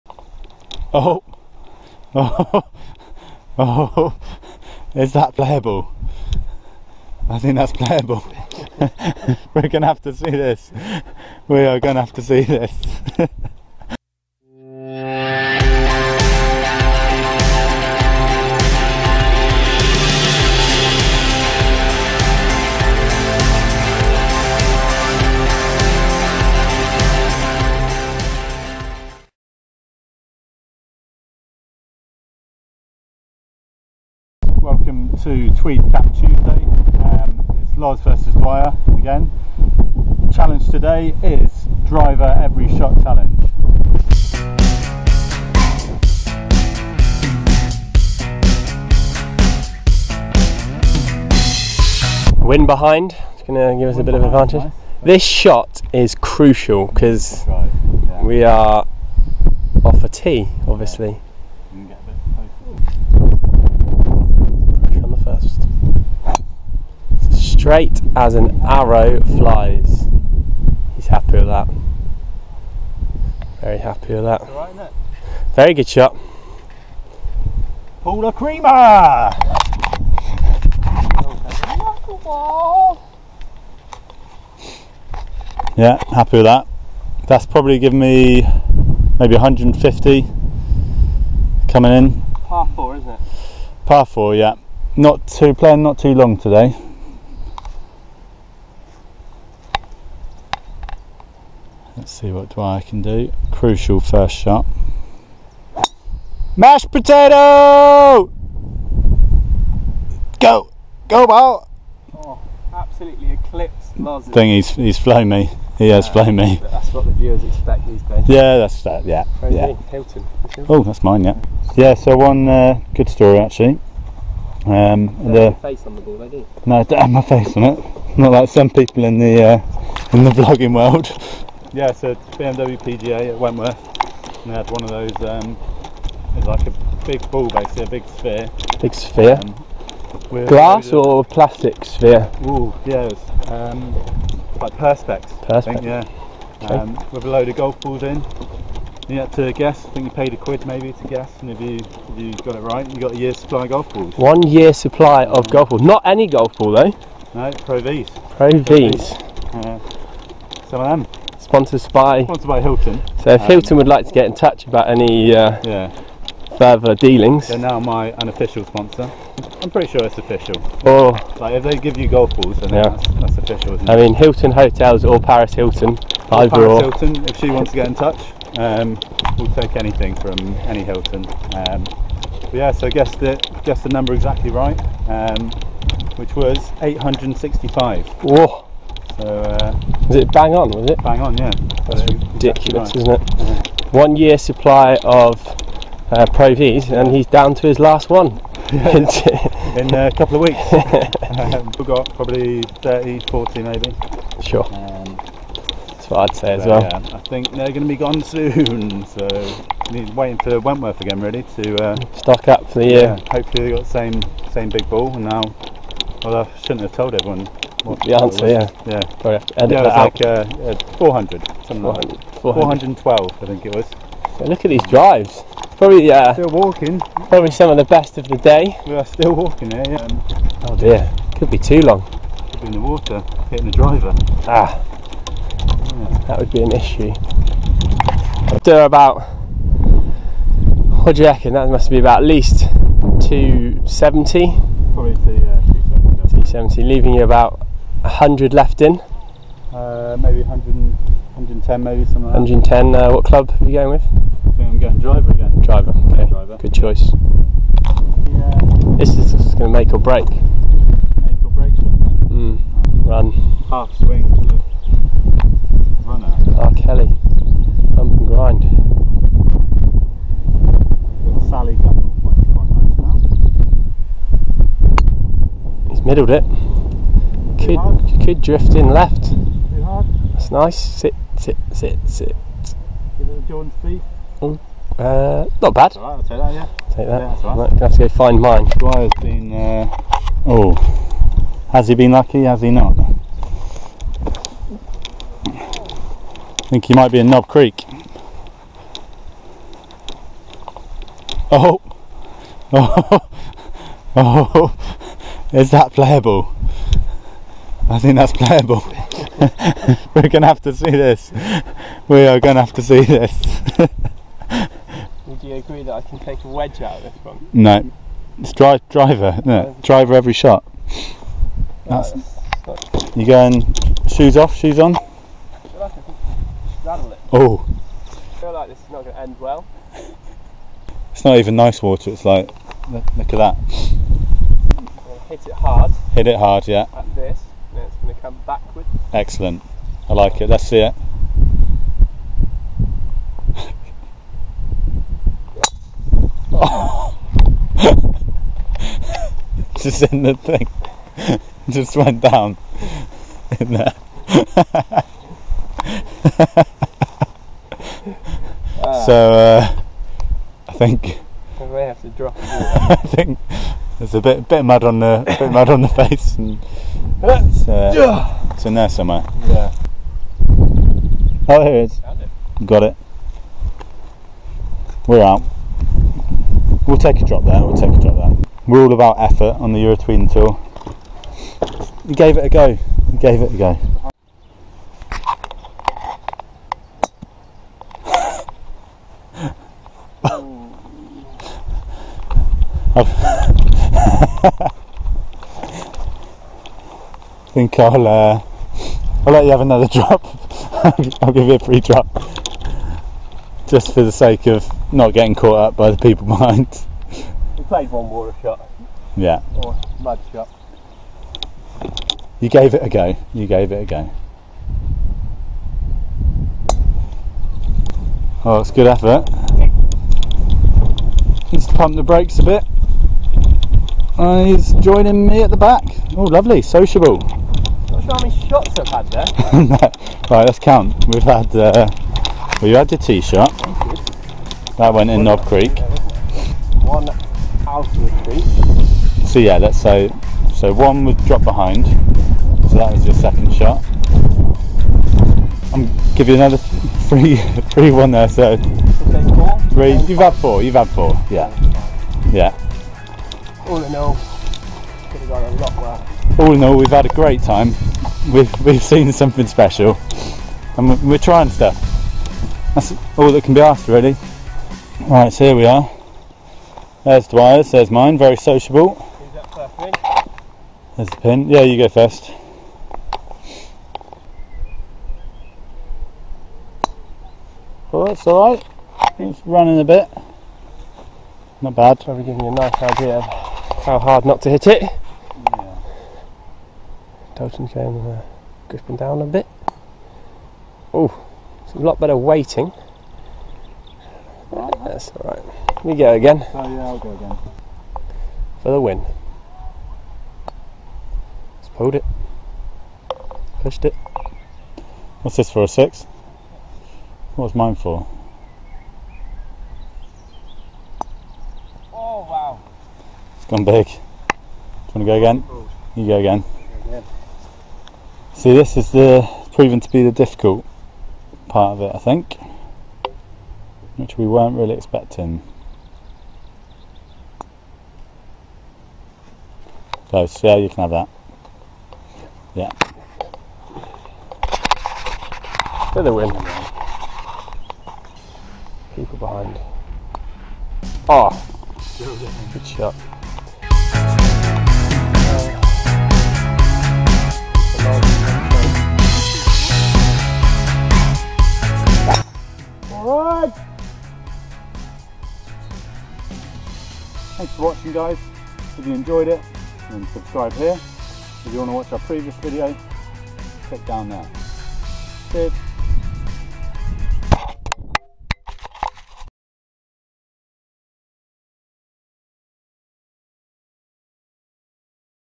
Episode 2: Driver Every Shot - A true test of a golfer!
It's Tweed Cap Tuesday again!

Episode 2 looks in to whether it's easier to just forget about picking a club, going straight for the driver instead.

Loz and Dwyer take on the test again. Who's your money on? Will we get a winner this week?

Keep up to date with all our shenanigans on the Eurotweedan Tour Instagram and twitter - @eurotweedantour.